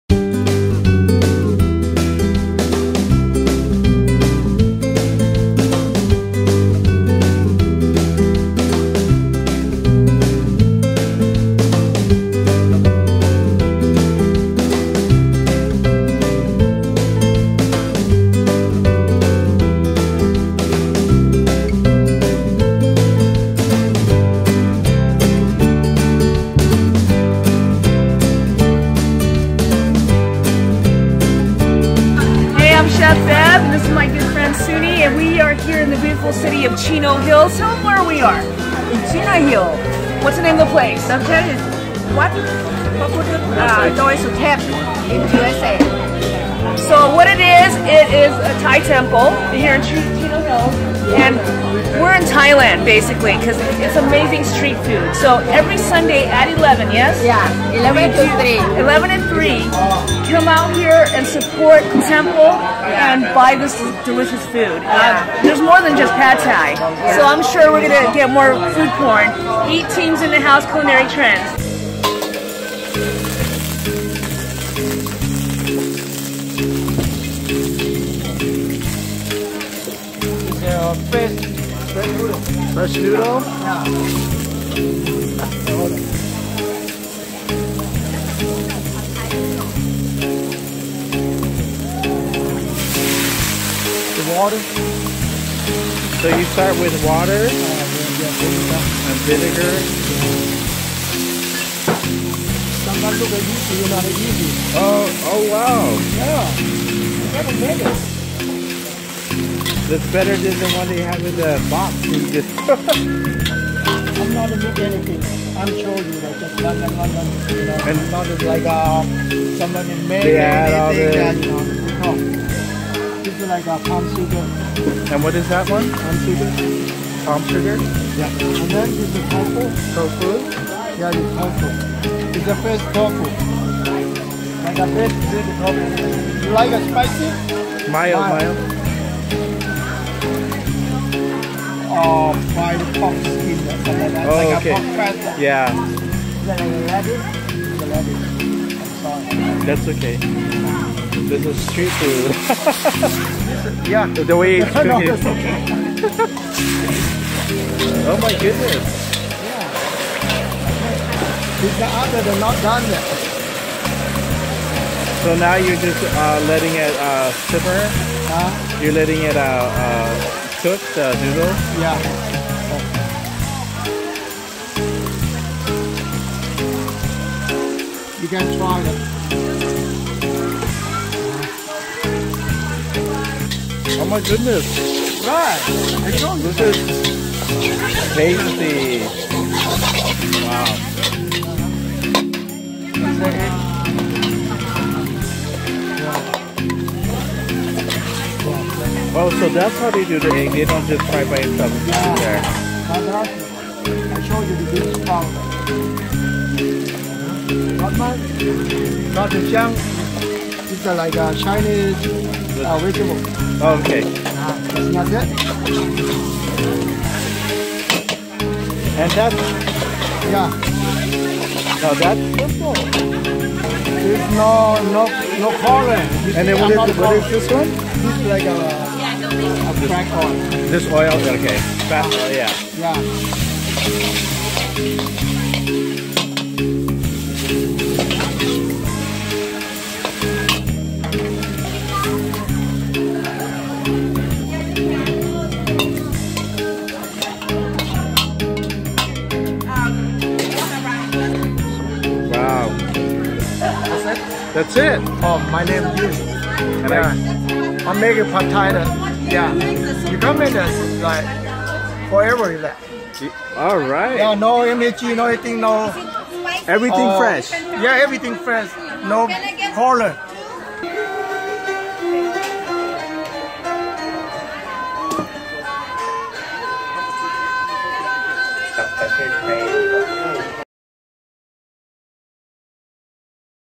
Foodie Trip Discovering Thai Food at Buddhist Temple
Chef Bev Lazo takes us on a Foodie Trip to a Thai Buddhist Temple - Wat Phrathat Doi Suthep USA in Chino Hills, CA.

Every Sunday from 11am to 3pm the temple opens up a Thai market place featuring various Thai Food and veggie stalls. The beauty of this "street food" marketplace is that proceeds from the sales goes to funds to help build a permanent temple.

Visitors are also welcomed into their temporary temple as monks chant and pray.